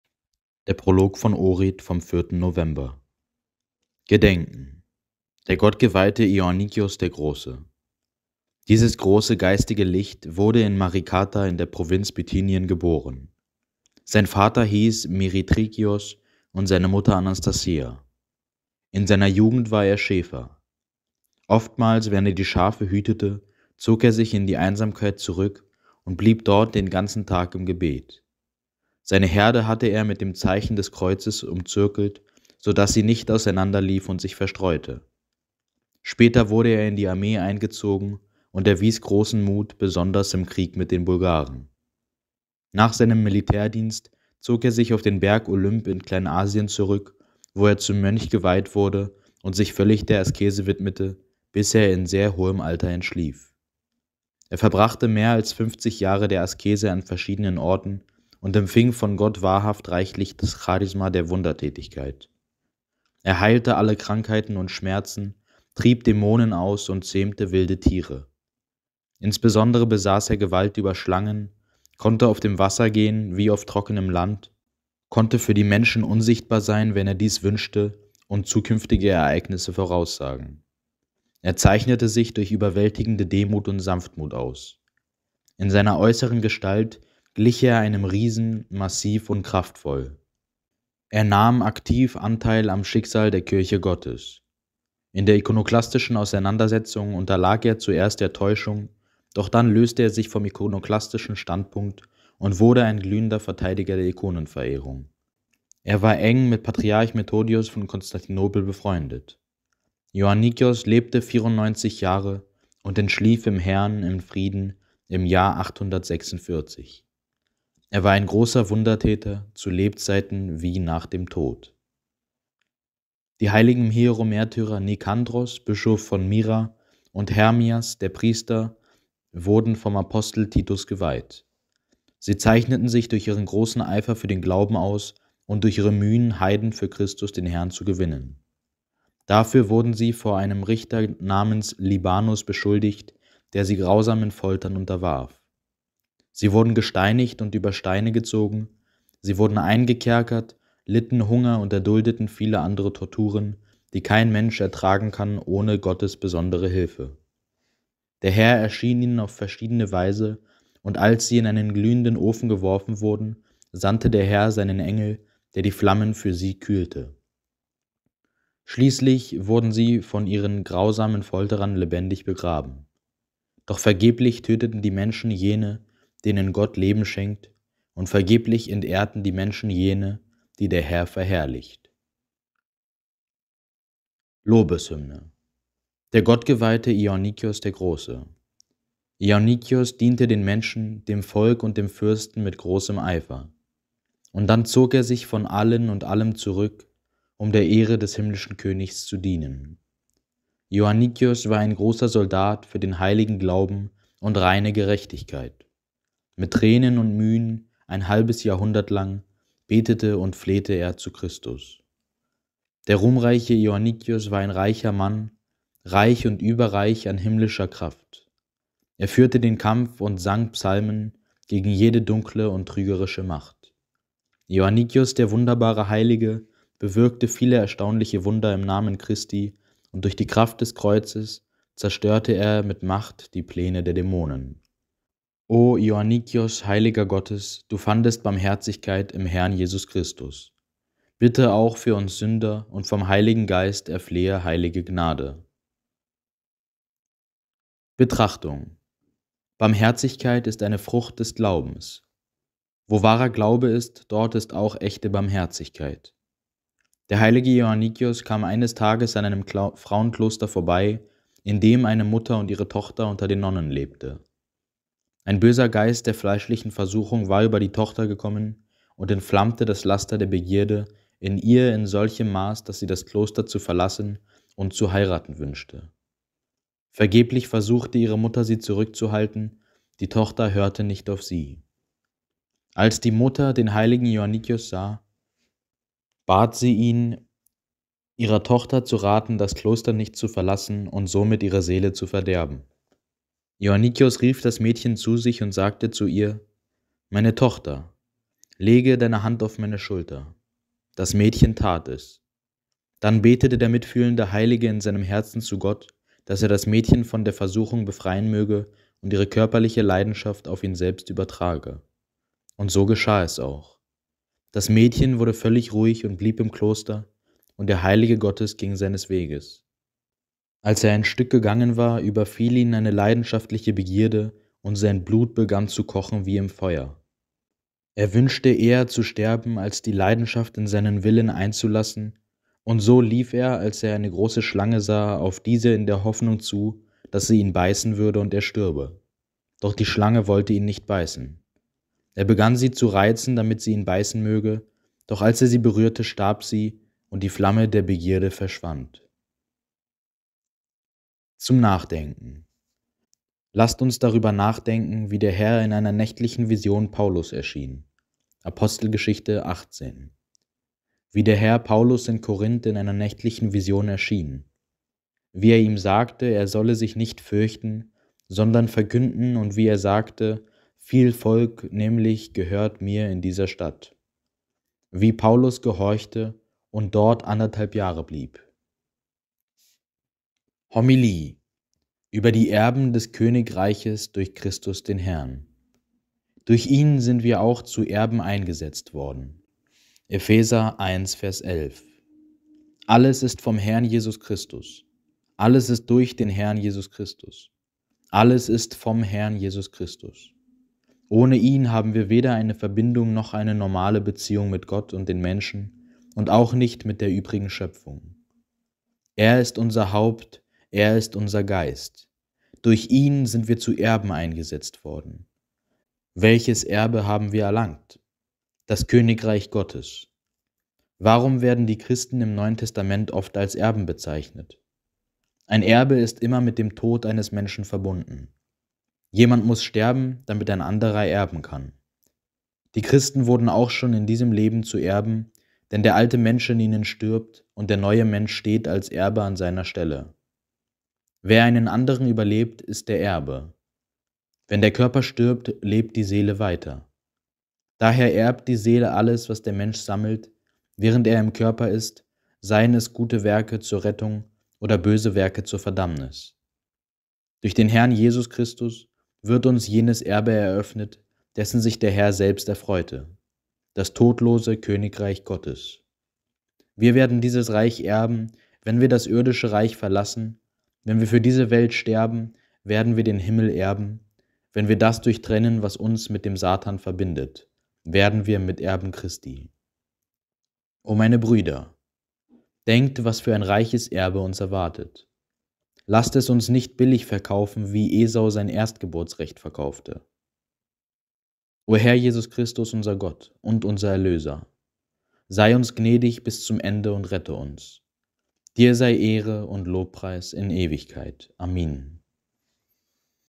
Der Prolog von Ohrid vom 4. (17.) November
Gedenken:
1. Der gottgeweihte Ioannikios der Große
2. Die hll. Hieromärtyrer Nikandros, Bischof von Myra, und Hermias, der Priester

Lobeshymne

Der gottgeweihte Ioannikios der Große

Homilie

Über die Erben des Königreiches durch Christus den Herrn

Durch Ihn sind wir auch zu Erben eingesetzt worden.

(Eph 1,11)
Hl. Nikolaj Velimirović. Der Prolog von Ochrid. ISBN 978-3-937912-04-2